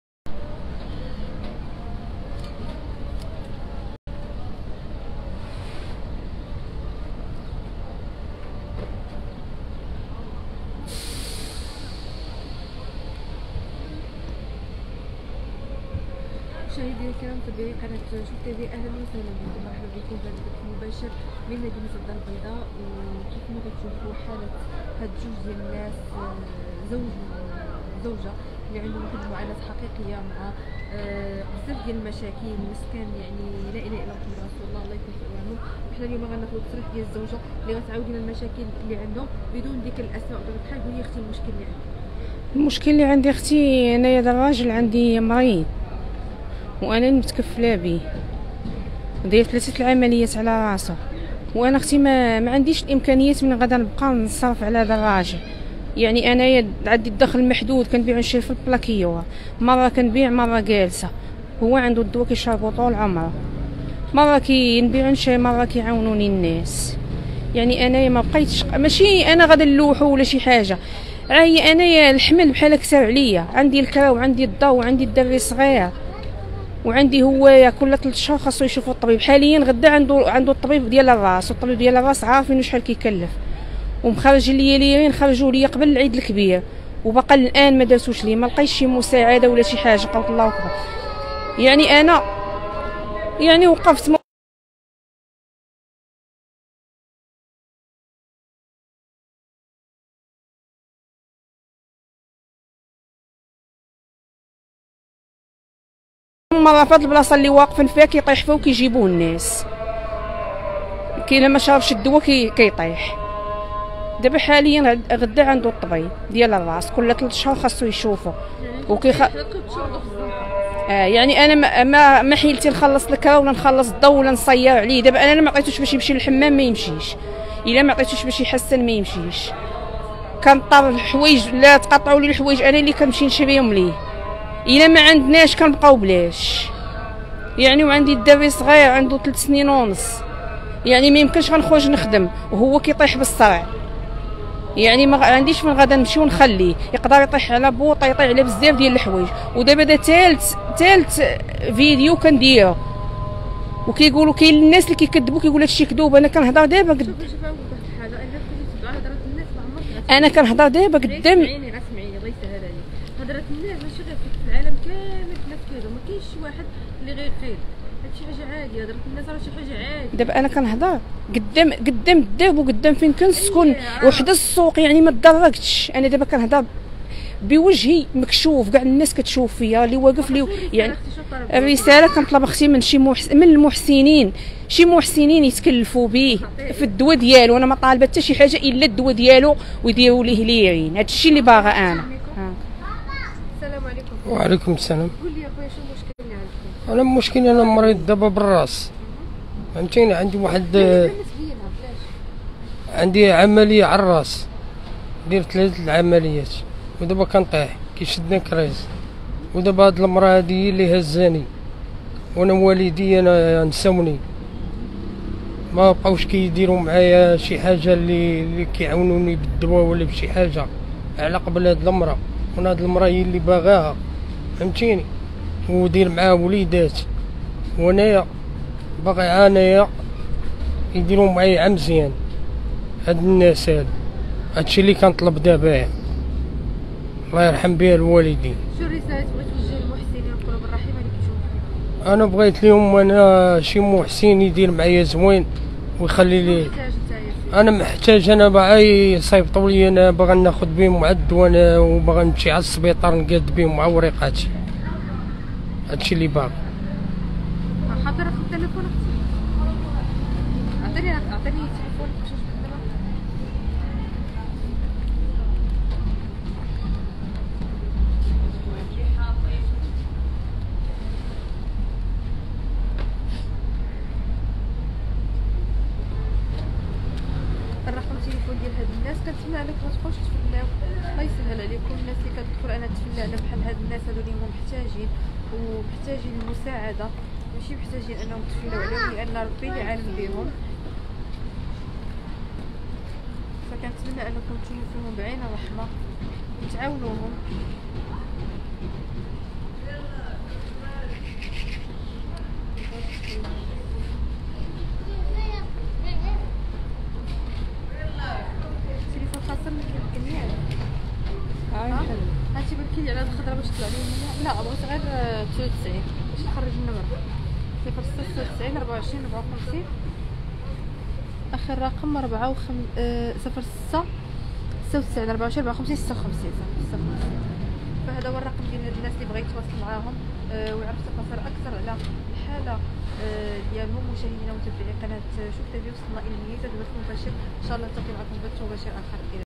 مباشرة من الدار البيضاء.. معاناة كوبل عايش في الزنقة ماعندهم لا حنين لا رحيم و راجلها مريض بزاف
مباشرة من الدار البيضاء.. معاناة كوبل عايش في الزنقة ماعندهم لا حنين لا رحيم و راجلها مريض بزاف و ملقات ليه جهد: بغيت لي يعاوني أنا و راجلي